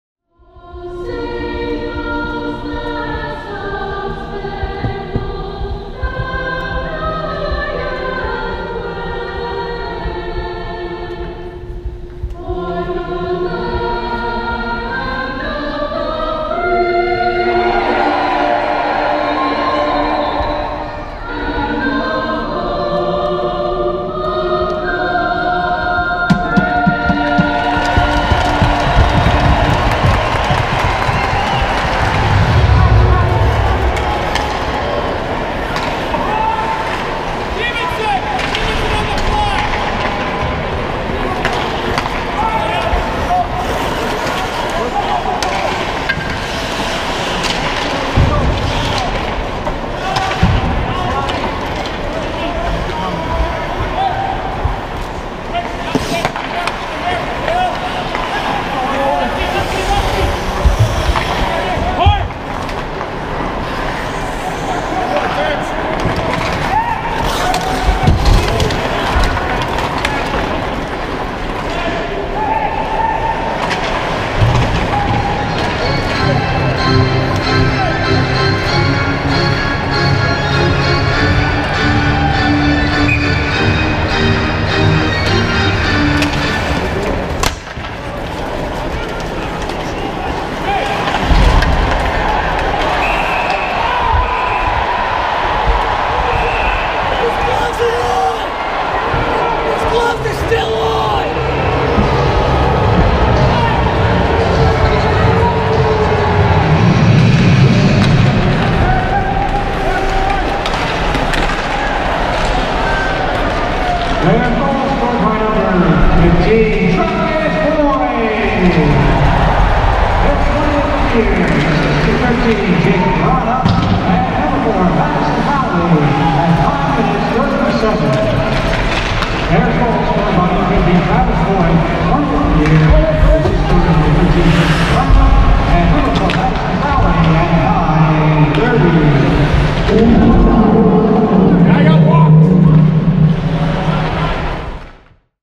Hershey Bears 360
A 360 look at a Bears game from between the benches.